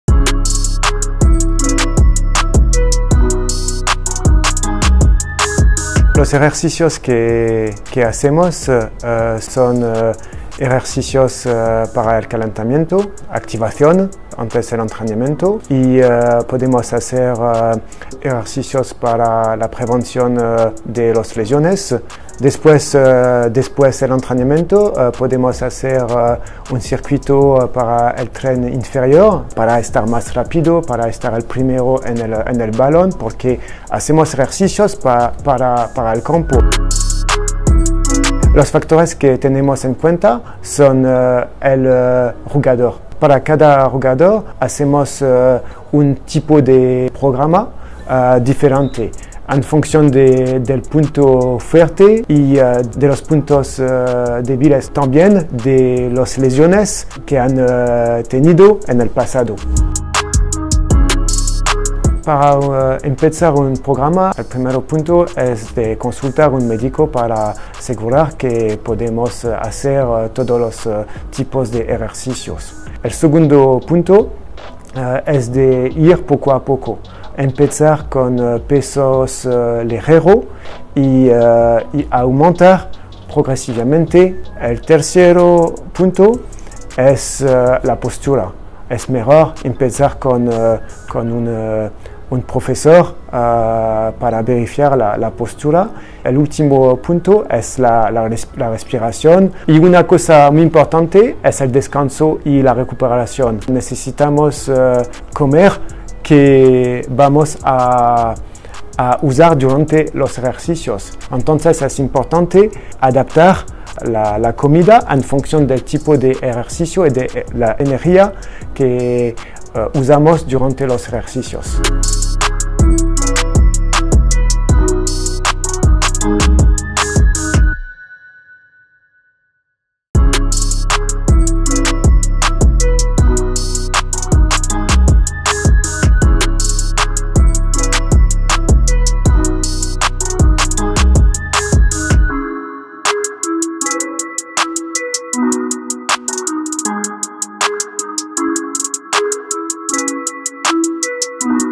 How are Real Madrid players' training routines planned?
Learn all about Real Madrid's day-to-day physical preparation thanks to the team's fitness coach, Grégory Dupont, and the support of Sanitas. Activation, warm-up, the lead up to training, injury prevention and specialised training programmes for each player, among other things. Dupont tells us all about how the team prepares.